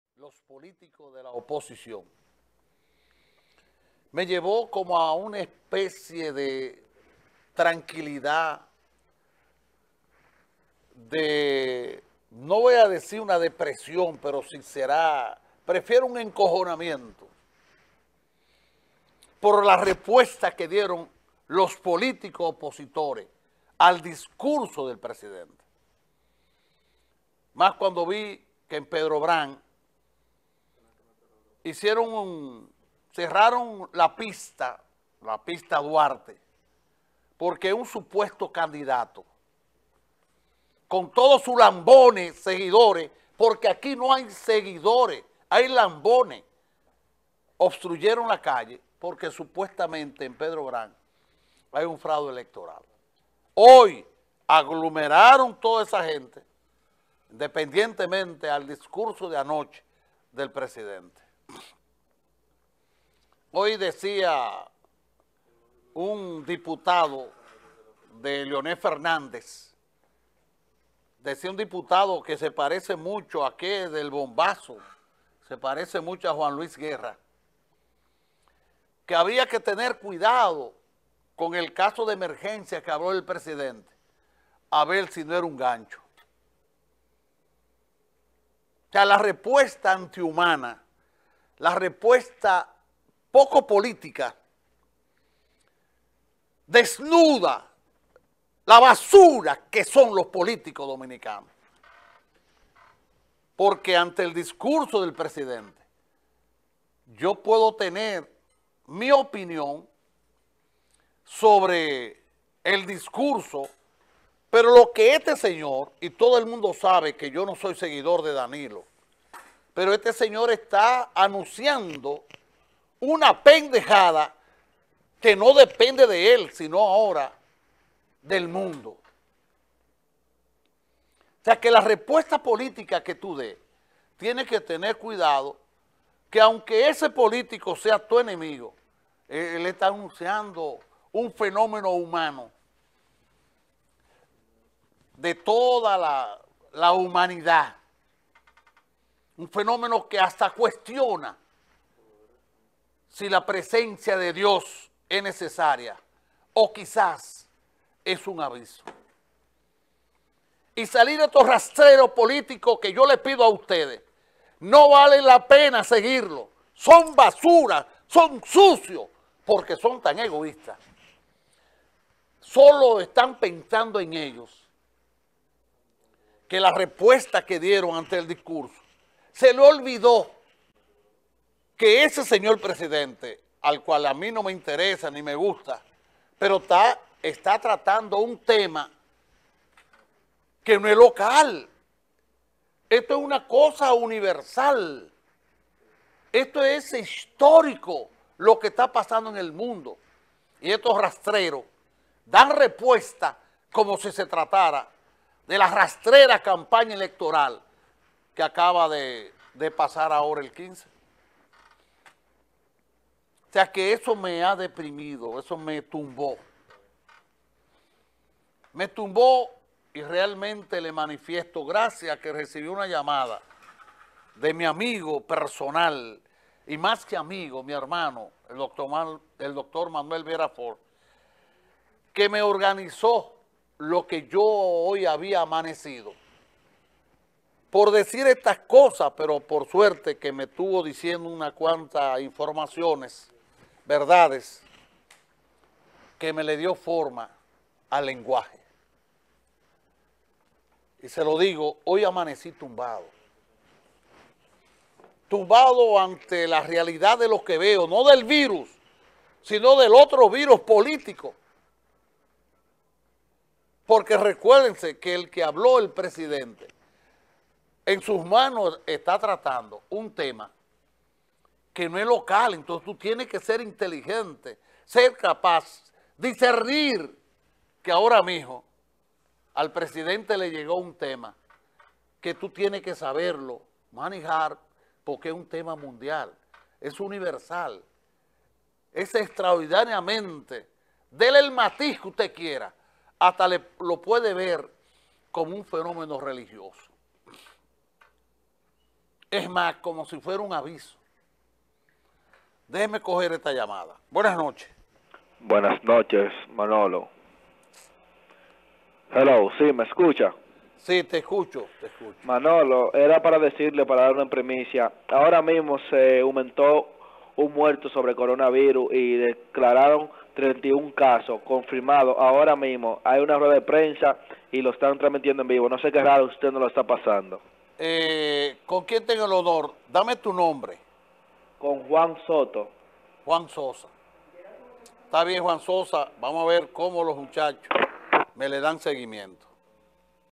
Políticos de la oposición dieron una respuesta anti-humana al discurso de Danilo
Políticos de la oposición dieron una respuesta anti-humana al discurso de Danilo según Manolo Bonilla